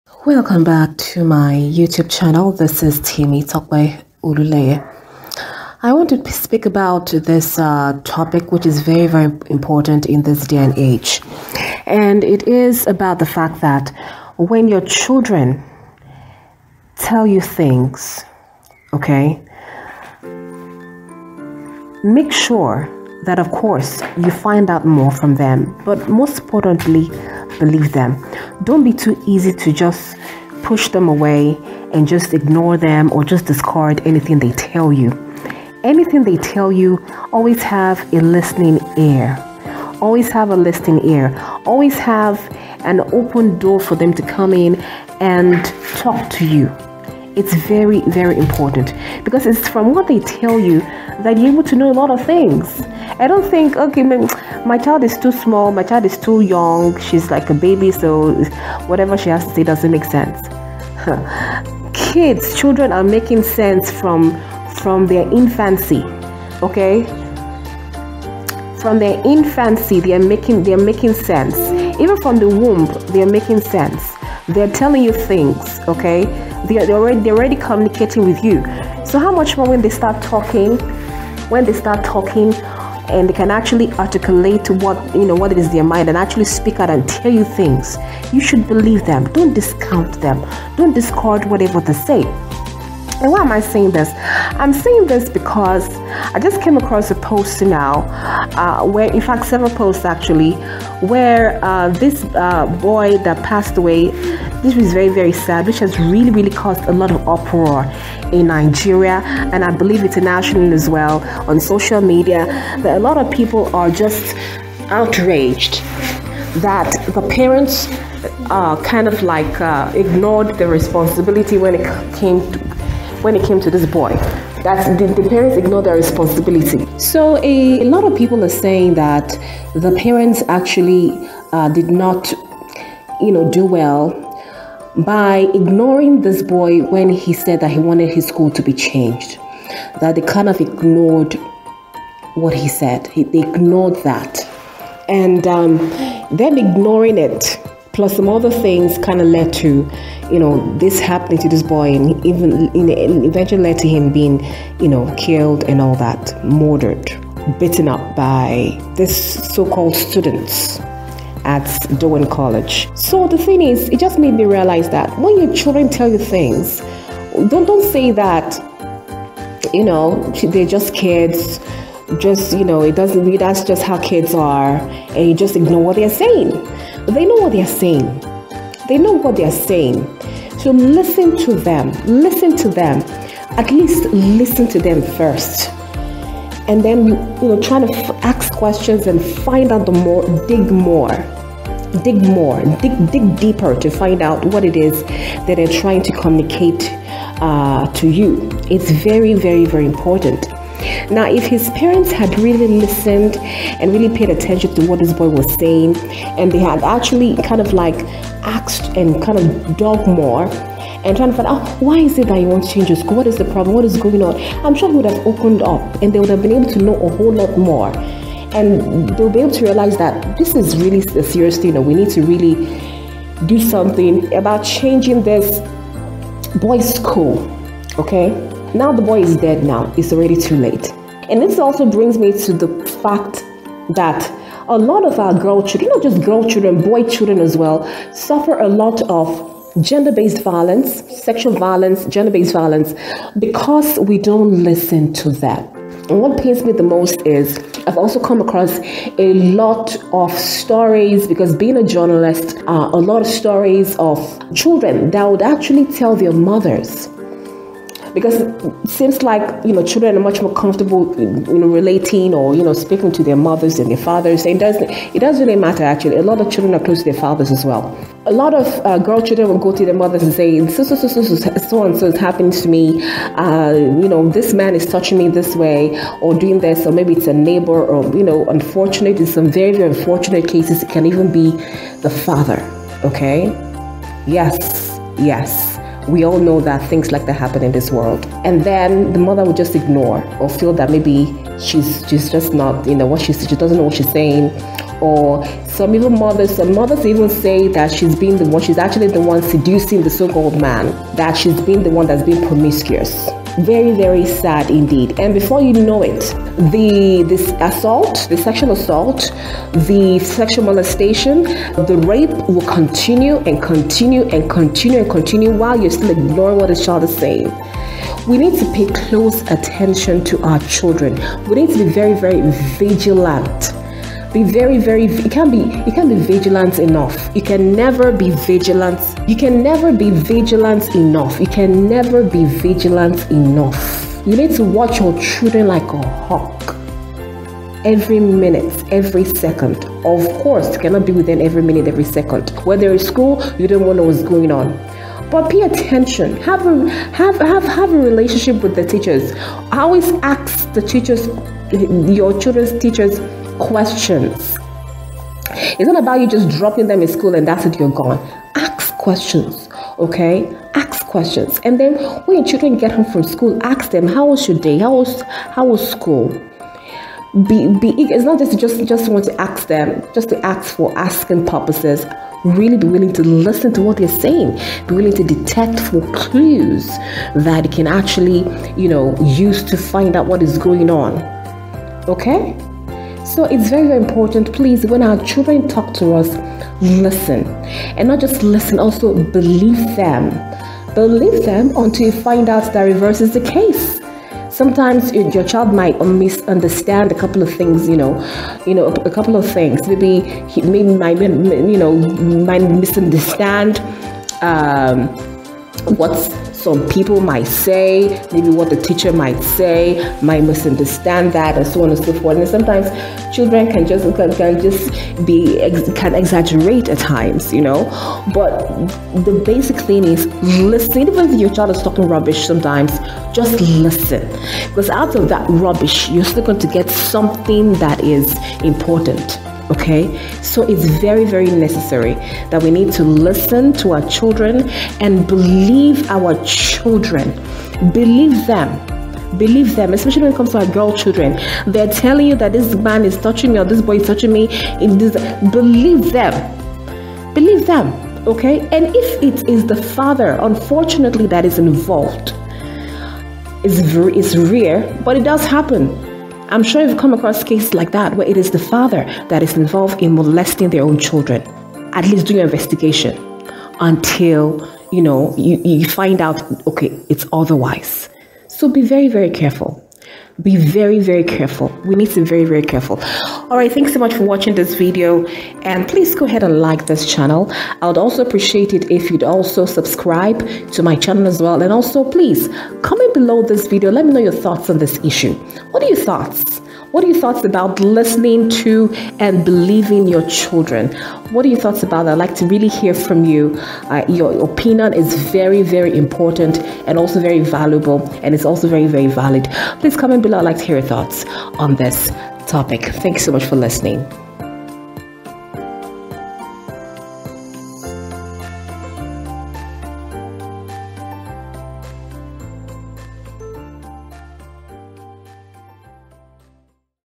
Parents, Please Listen To Your Children & Believe Them | What Late Sylvester Oromoni Said
In this video, I talk at length about how important it is for parents to always listen to their children and believe them. I talked about the inherent dangers in not doing so.